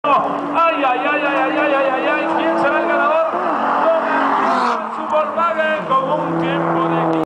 2008-07-03 Golf VR6 vs Xsara Rojo
Pikes en Texeira
Jueves, 3 de julio de 2008.

VW Golf VR6 Crema  vs  Citroen Xsara Rojo